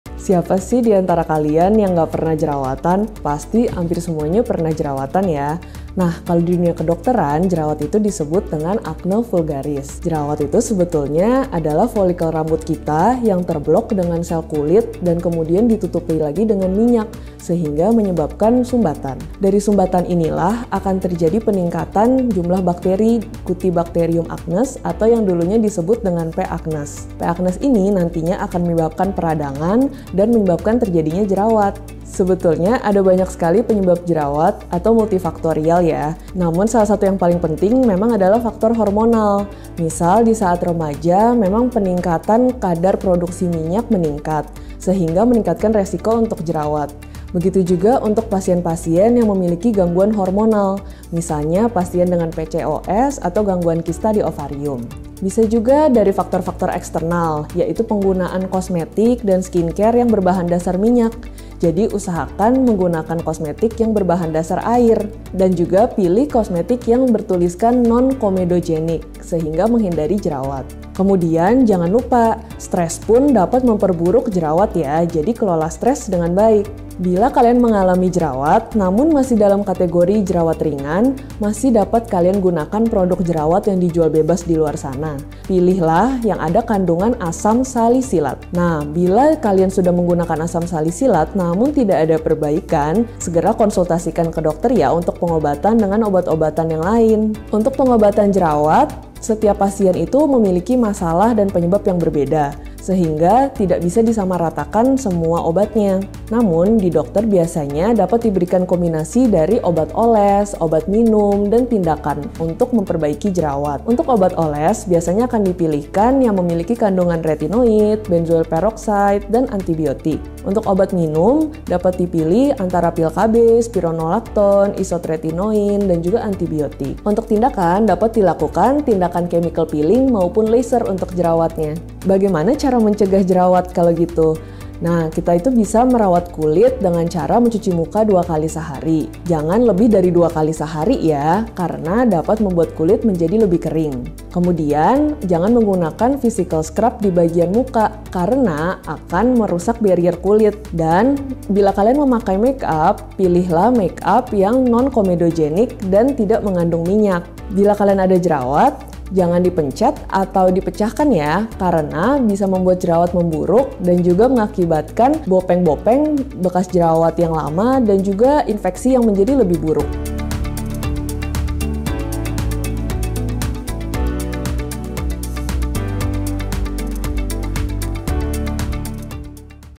Jerawat: Penyebab, Gejala, Pengobatan, dan Pencegahan
Kerap dianggap sebagai salah satu ancaman pada kulit, jerawat dapat terjadi karena beberapa hal. Jerawat tidak hanya dapat muncul di wajah, namun juga bisa muncul di area leher, dada, dan punggung. Yang menjadi pertanyaan, yaitu adakah cara efektif untuk mengatasi dan mencegahnya? Temukan jawabannya di video ini.

00:01 - Penyebab Jerawat
01:17 - Pengobatan Jerawat
02:13 - Pencegahan Jerawat
Aplikasi Halodoc: Chat dengan Dokter, Toko Kesehatan, Buat Janji RS, dan Janji Medis